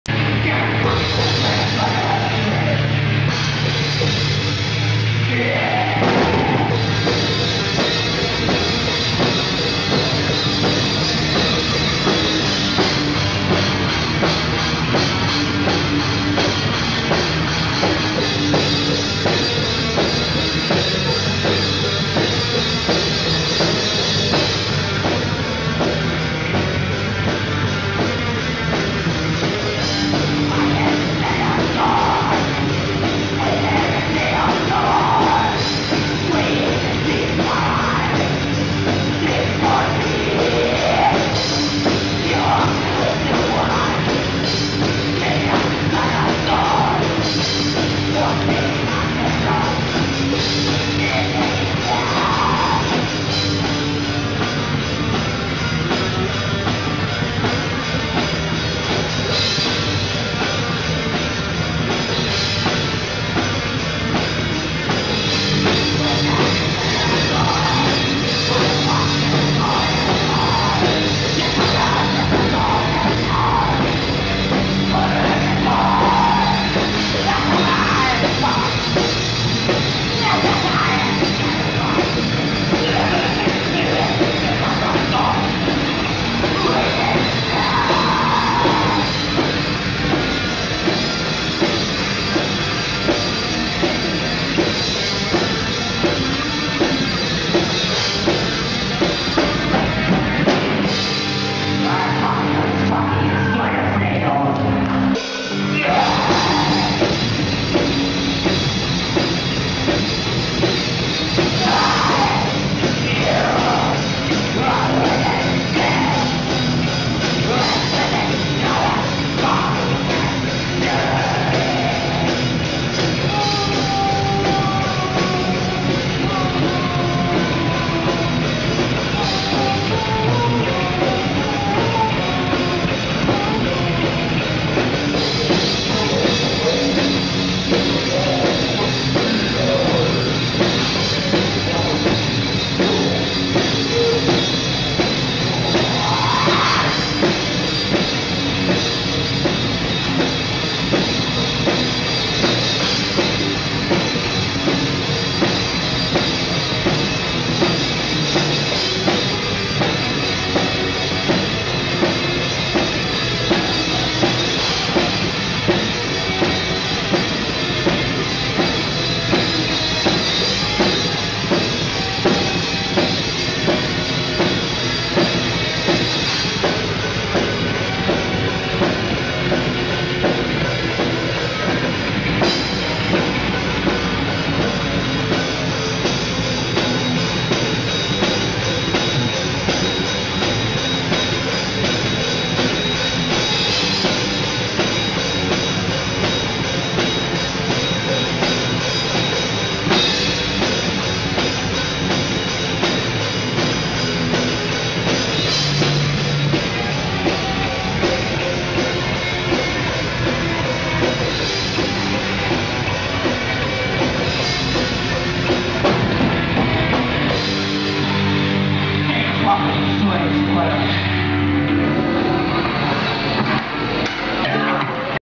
modar black metal temanggung
video Modar Black Metal dr Temanggung,.yg di vokalisi oleh MBAHNE,.pokok'nya tetep Kejem,Sangar,.
MODAR DE,.!!!